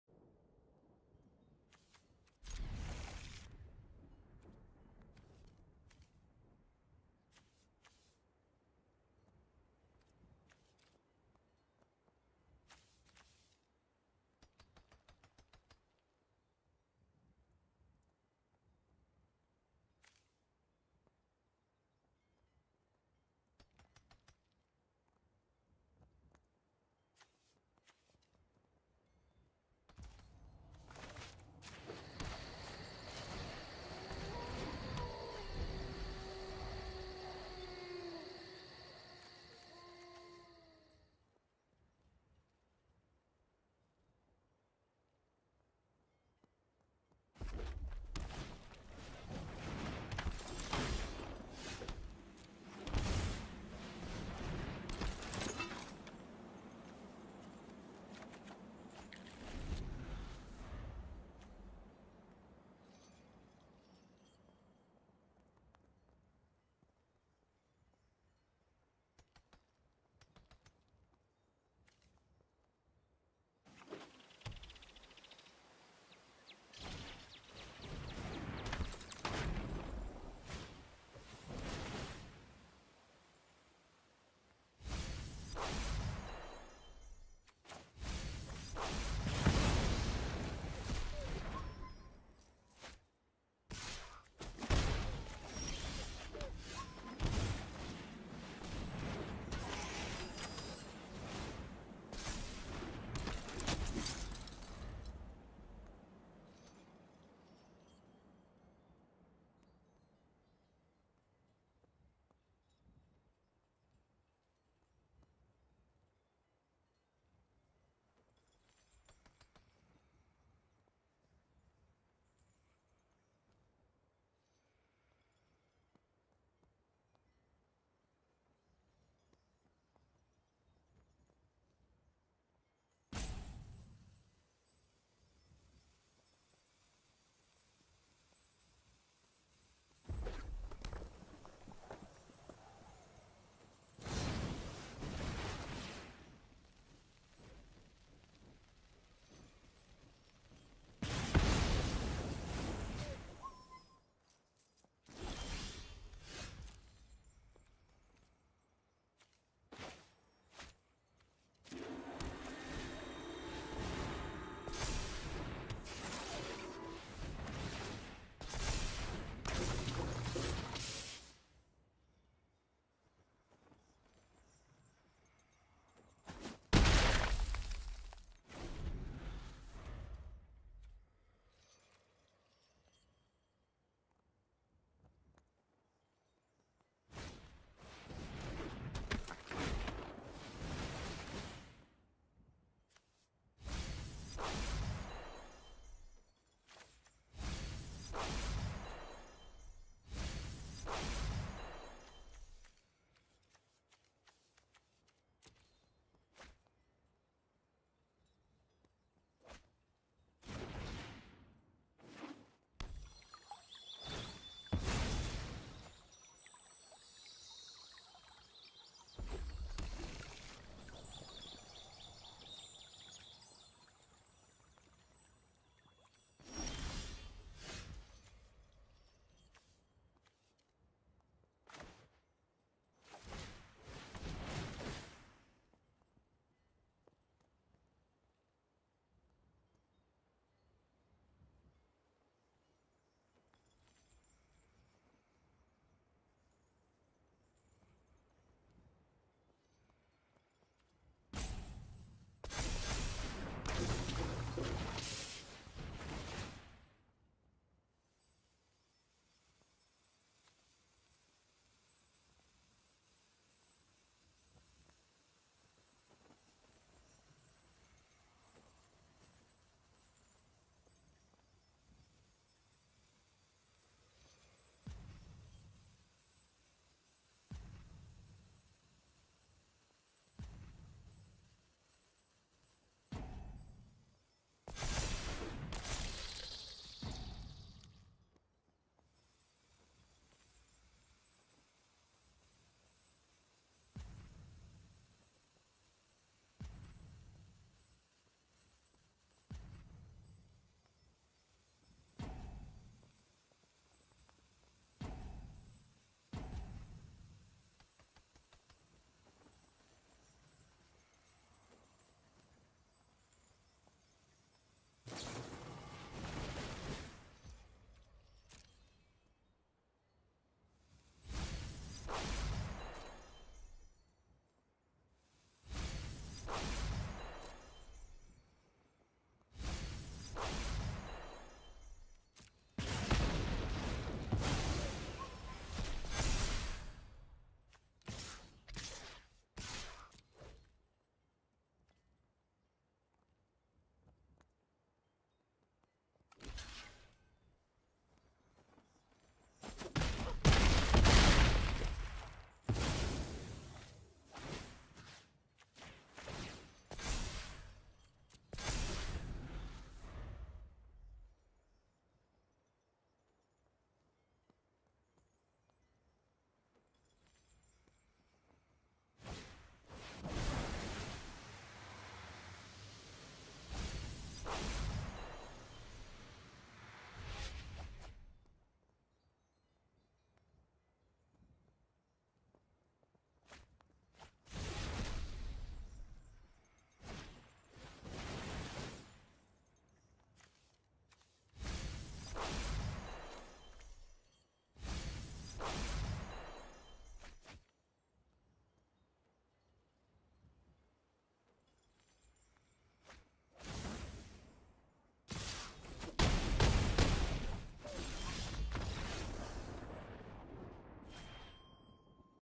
Scaley Boiz vs Delver/Phoenix
The new Patchwork Automaton is pretty nutty for a Hardened Scales/Modular deck.  Hard to target.